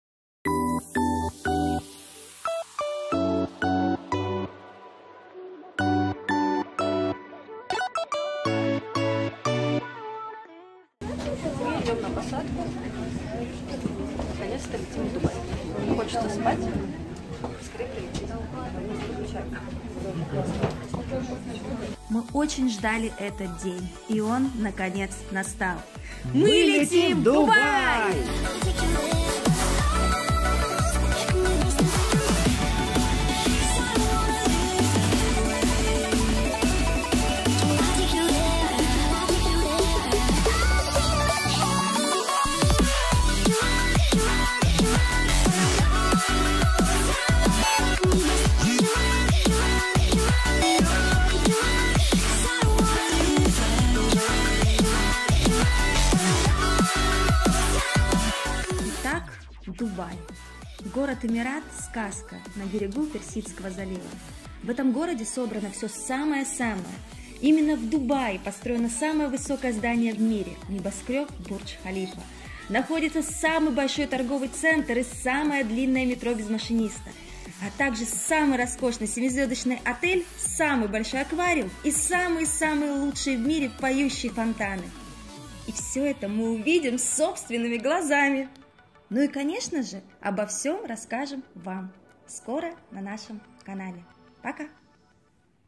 Дубай - город мечты! Наши первые впечатления! Отдых в ОАЭ Vlog
Мы начинаем серию выпусков про Дубай! Нас ждет интересное путешествие и отличный отдых! Оставайтесь с нами! И узнаете как лучше всего провести время в этом прекрасном городе!

И МНОГОЕ ДРУГОЕ НАЙДЕТЕ НА НАШЕМ КАНАЛЕ!

НАШ БЛОГ: kudapoehat.blog